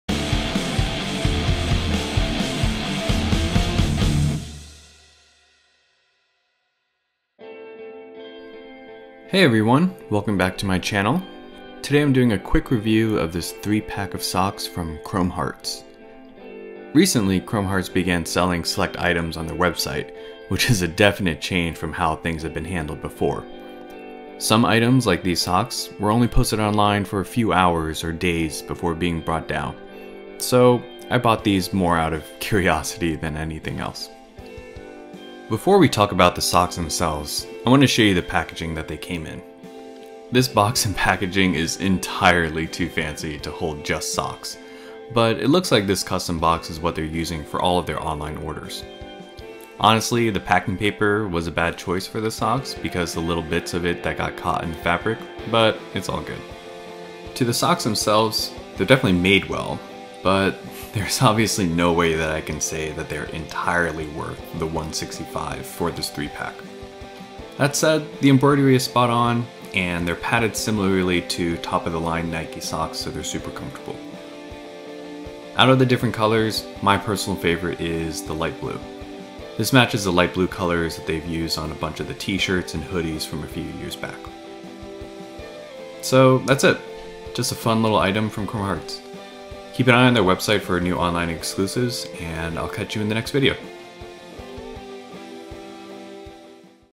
[review] CHROME HEARTS socks
Chrome Hearts has started to randomly release items on their website, so here's a quick video of the socks that they sold recently.

PLEASE no questions on exact pricing for any CH products on any of my videos, thanks!

If you haven't already, read through these articles for more background of the brand itself: